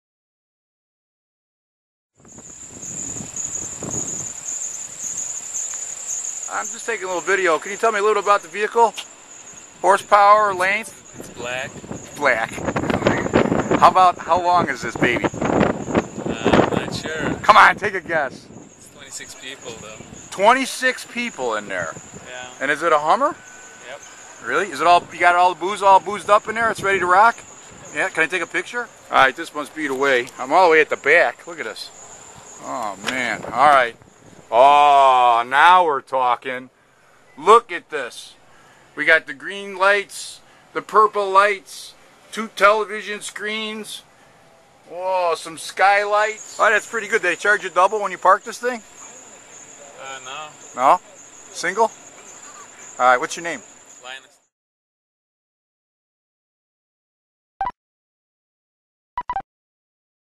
The Limo
A short video of the The Limo used for the wedding party